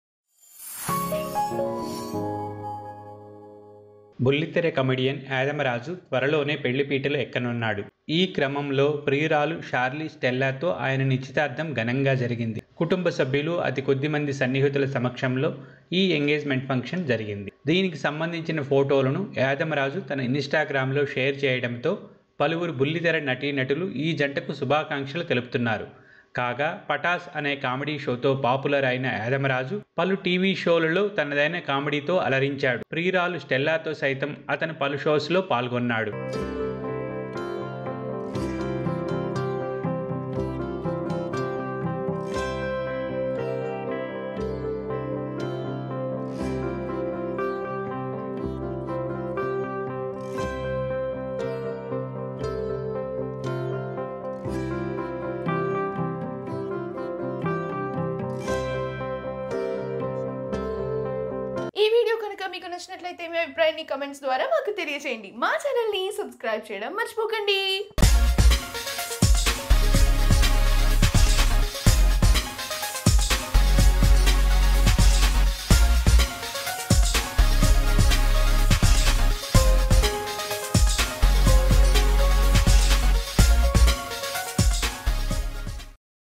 Patas Comedian Yadamma Raju & Stella engagement Photos||పటాస్ కమెడియన్ యాదమరాజు ఎంగేజ్మెంట్ ఫొటోస్
Patas fame comedian Yadama Raju and Stella gets engaged